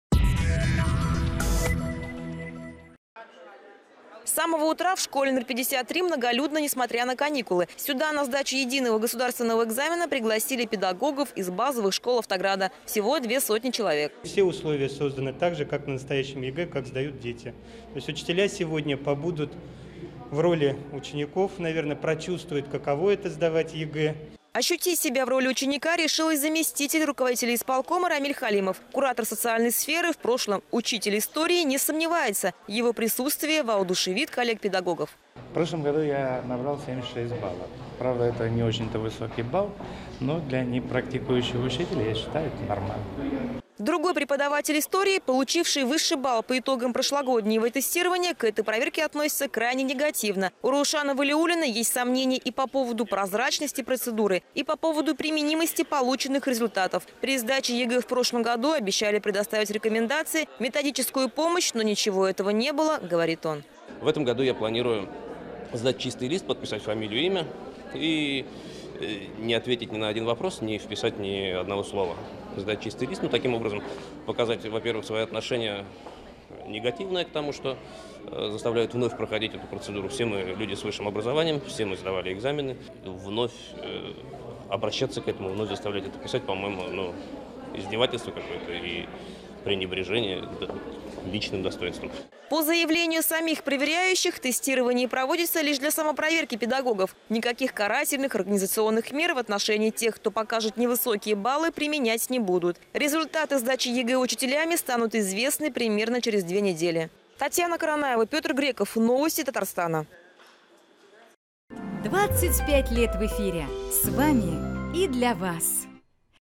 Челнинские учителя протестуют против сдачи ЕГЭ
Преподаватели решили сдать комиссии пустые бланки. Массовая проверка учителей прошла в школе № 53.